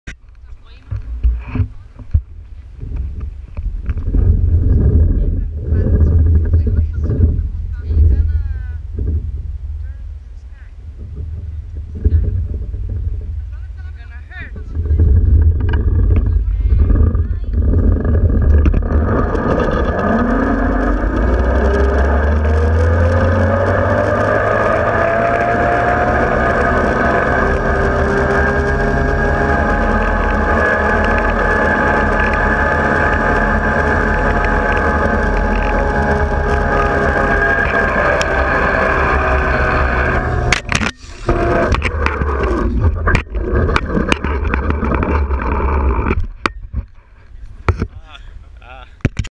Flying Fox at Dipotama
250 meters Flyng Fox at Dipotama during our Jeeping Full Day in Wild Evritania.